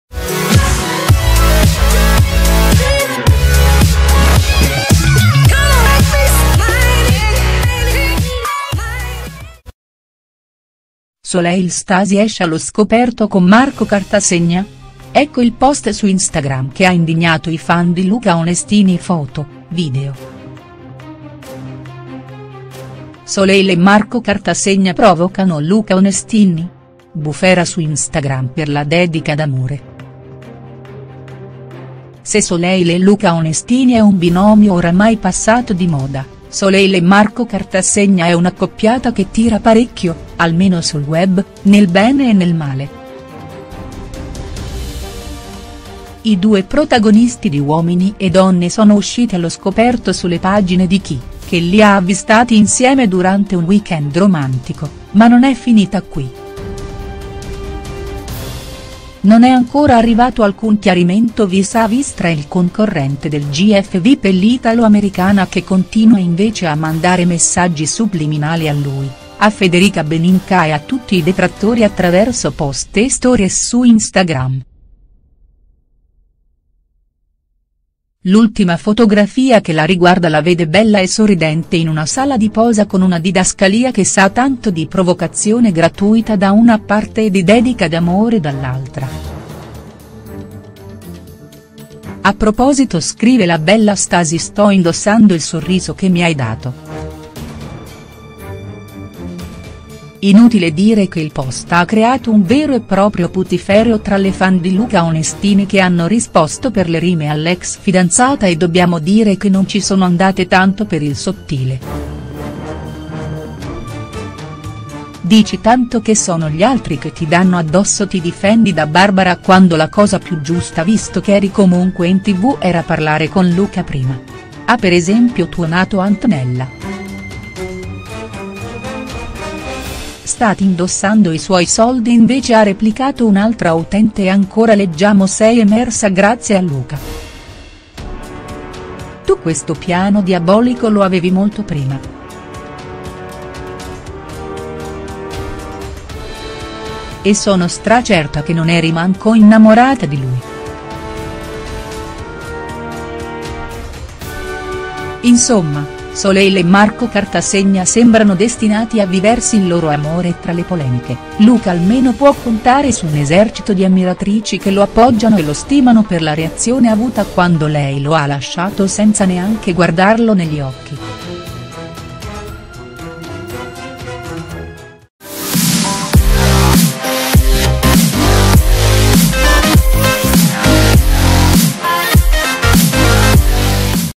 Soleil Stasi esce allo scoperto con Marco Cartasegna? ...| STARS NEWS
Soleil Stasi esce allo scoperto e svela a tutti con un post su Instagram la verità sulla sua relazione con Marco Cartasegna. L'indignazione dei fan di Luca Onestini è evidente tanto da inondare di insulti il post di Soleil. Ecco la foto e le reazioni al post.